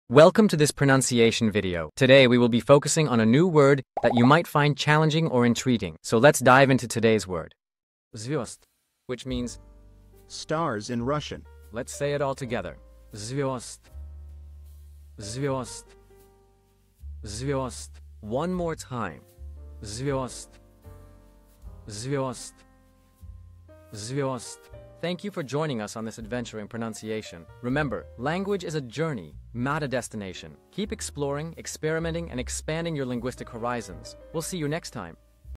How to pronounce Zvezd | Звезд (Stars in Russian)
Master the Pronunciation of 'Zvezd | Звезд' - which means : Stars in Russian 📝 with @PronunciationRussian 🎓🗣️ - [Your Guide to Russian]

Learn to pronounce 'Zvezd' perfectly in Russian with our easy and engaging tutorial by @PronunciationRussian! 📖📽️ In this comprehensive video, we provide you with native speaker examples 🗣️👂 and phonetic breakdowns 🎵 to ensure you get the pronunciation just right.